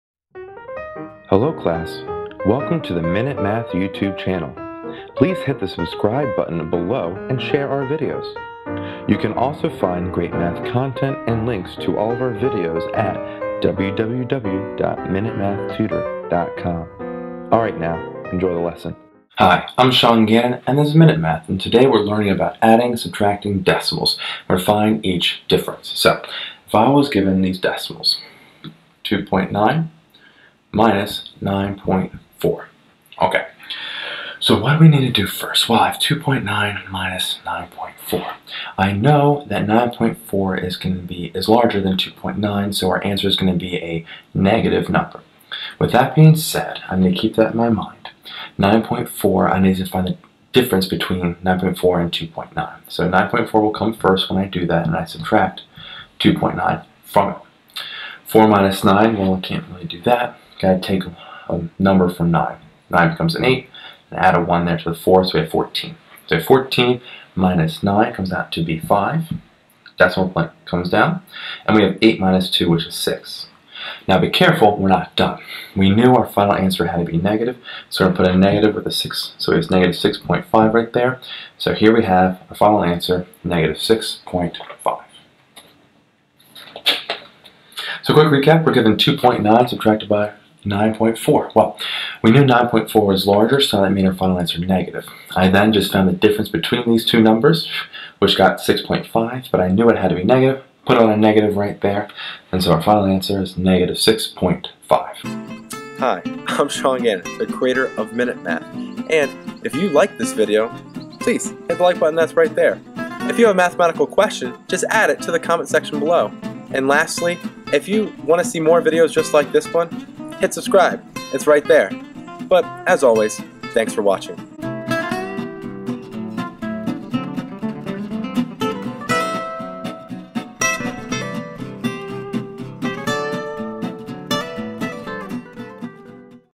Find the Difference 2.9-9.4
A math video lesson on Adding and Subtracting Decimals. This video evaluates 2.9-9.4